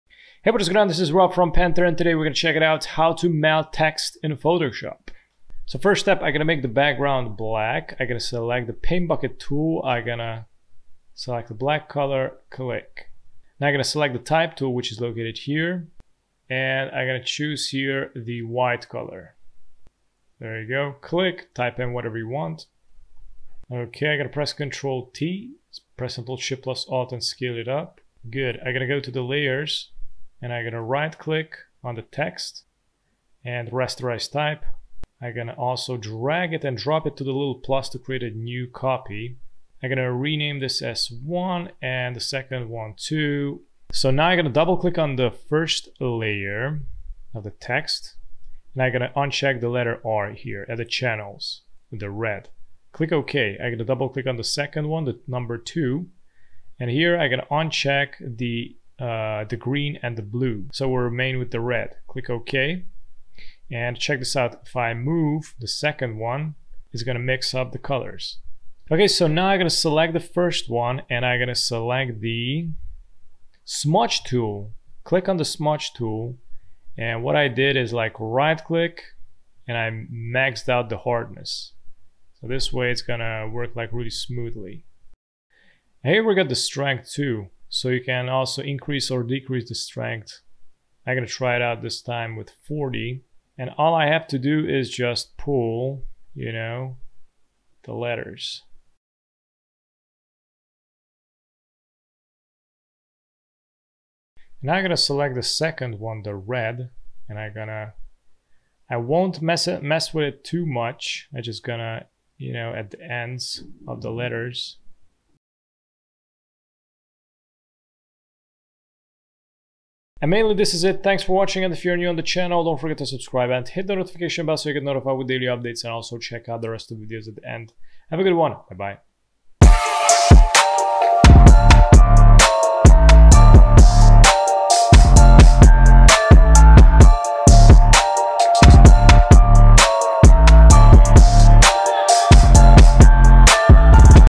Liquid Text Effect - Adobe Photoshop
I tried another liquid effect with a different technique that get's you a little less flexibility because we only work with he RGB channels in the layer properties. In this example I will show you a 2 color variation example that is simple and effective.